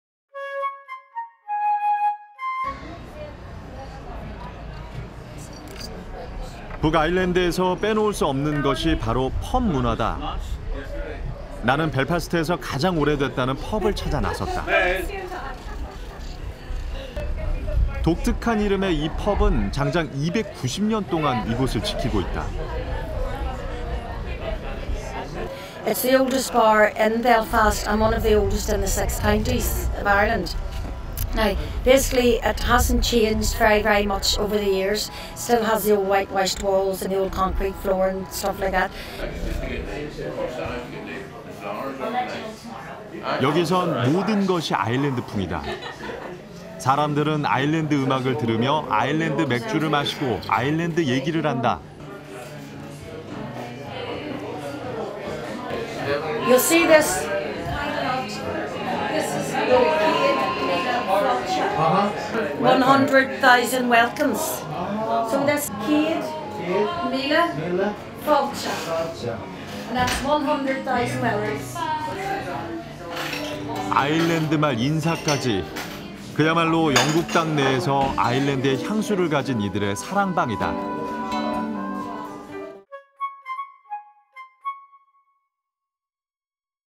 【K】UK Travel-Belfast[영국 여행-벨파스트]가장 오랜된 펍, 캘리의 지하창고/Pub/Kelly'S Cellars/Beer
[한국어 정보]
북아일랜드에서 빼놓을 수 없는 것이 바로 ‘펍 Pub 문화’이다. 나는 벨파스트에서 가장 오래됐다는 펍을 찾아 나섰다. 독특한 이름의 이 펍은 장장 290년 동안 이곳을 지키고 있다. “켈리의 창고는 벨파스트에서 가장 오래된 바입니다. 전체 6개 주 중에도 가장 오래, 예전 모습 그대로 유지. 흰 벽, 콘크리트 바닥 등 그대로.” 여기선 모든 것이 아일랜드 풍이다. 사람들은 아일랜드 음악을 들으며 아일랜드 맥주를 마시고 아일랜드 얘기를 한다. “이 벽에 적힌 글자를 보세요. 십만 번 환영합니다.” 아일랜드말 인사까지. 그야말로 영국 땅 내에서 아일랜드에 향수를 가진 이들의 사랑방이다.

The book is an essential right in the Irish 'Pub Pub Culture ". I went looking for the oldest pub in Belfast dwaetdaneun. This pub is keeping this place a unique name for the epic 290 years. "Warehouse of Kelly's oldest bar in Belfast. Full longest during six weeks, the old appearance intact. Stay up white walls, concrete floors, etc. "Everything in here is the Irish style. Irish people are drinking beer while listening to Irish music and the Irish story. "Look at the letters written on the wall. Welcome hundred thousand times, "Irish by the end of Personnel. Indeed the UK Sarangbang with their perfume in Ireland within the earth.

[Information]
■클립명: 유럽110-영국06-08 가장 오랜된 펍, 캘리의 지하창고/Pub/Kelly'S Cellars/Beer
■여행, 촬영, 편집, 원고: 이영준 PD (travel, filming, editing, writing: KBS TV Producer)

[Keywords]
,유럽,Europe,유럽,영국,United Kingdom,United Kingdom,UK,이영준,2011,6월 June,북아일랜드,Northern Ireland,Northern Ireland,